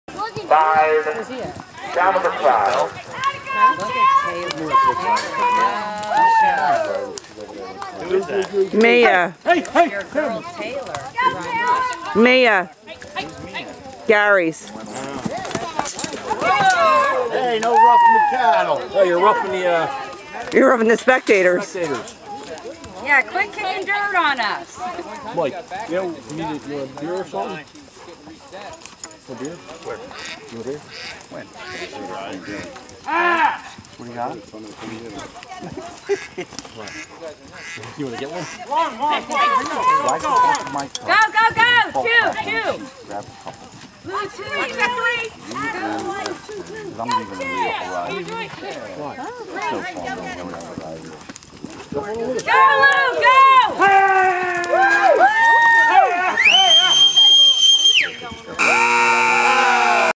Lou & Taylor Doin Work Ranch Sorting!!
My second time riding Mia had a blasttt:))